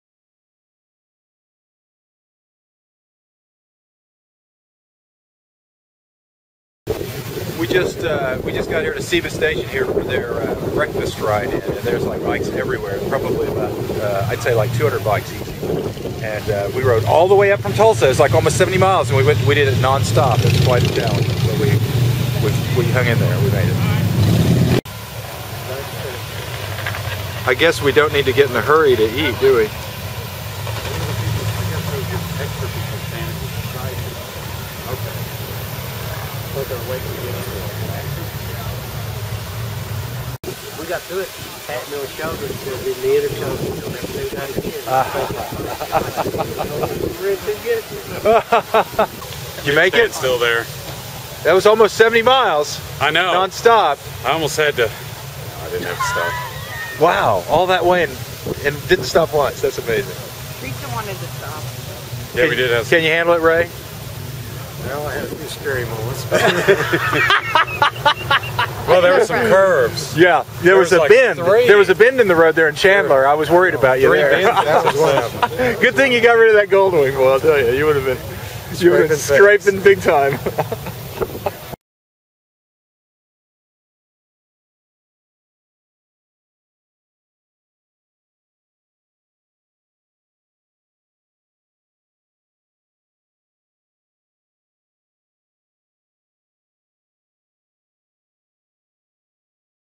Arrival at Seaba Station
First breakfast ride-in at the Seaba Station motorcycle museum on Route 66.